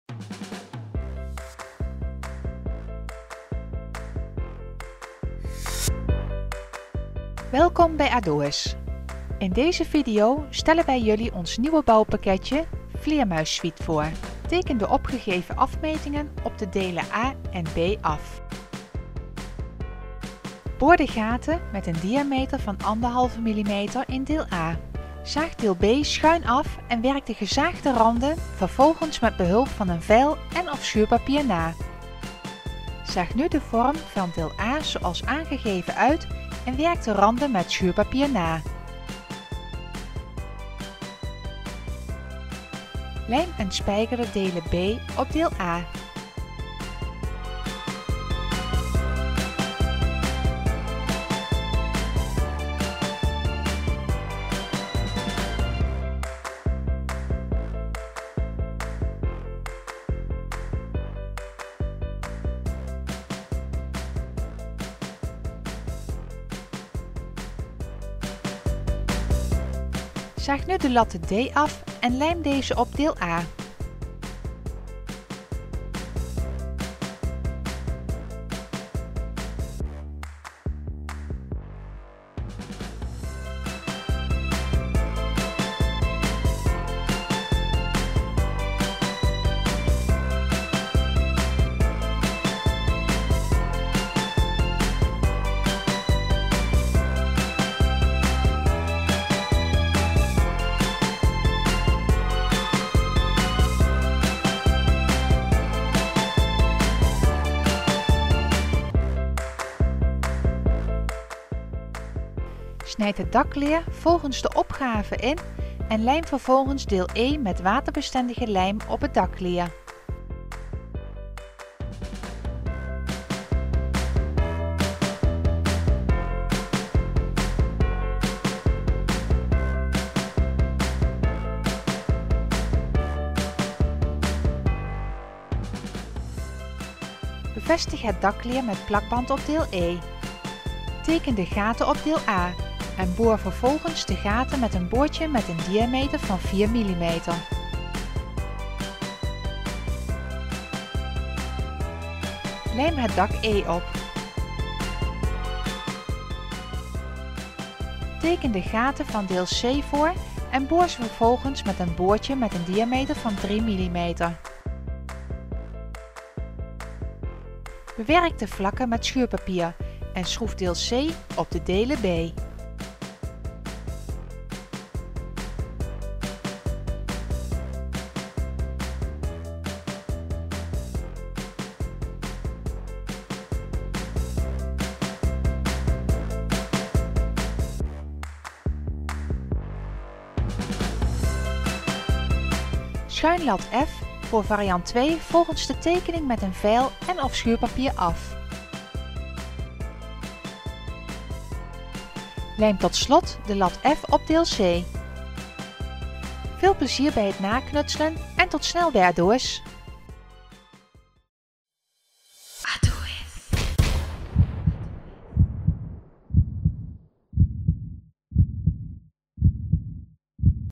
Zelf een vleermuiskast bouwen DIY handleiding
In deze video instructie laten wij zien, hoe eenvoudig en snel en een functionele vleermuissuite ontstaat. In dit bouwpakketje van Aduis worden de materialen hout en dakleer gebruikt. Het bouwpakketje, de bouwhandleiding, de benodigde gereedschappen, materialen en vele andere creatieve ideeën vindt u op de homepage van Aduis
Veel plezier!